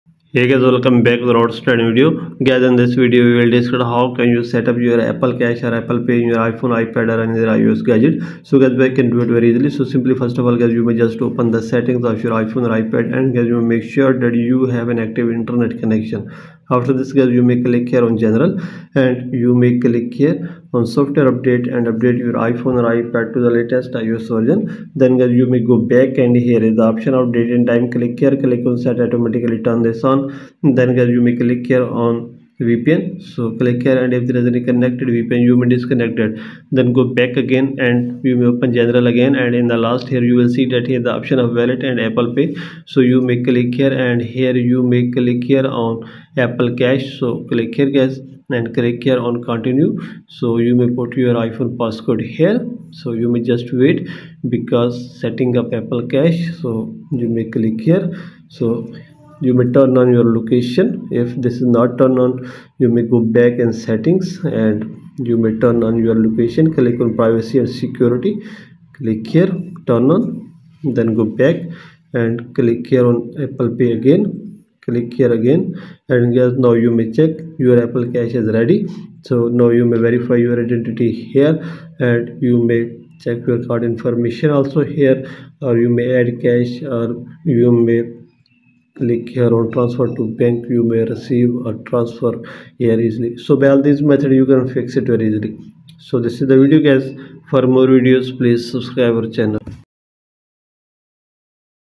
How To Set Up Apple Cash | How To Set Up Apple Pay in iPhone
Want to know how to set up Apple Cash? In this video, I'll guide you through the process of setting up Apple Cash on your iPhone, so you can send, receive, and spend money easily. Follow these steps to get started with Apple Cash today.

- Requirements for setting up Apple Cash
- Step-by-step guide to enable Apple Cash on your iPhone
- How to link your bank account or debit card
- Tips for using Apple Cash securely and efficiently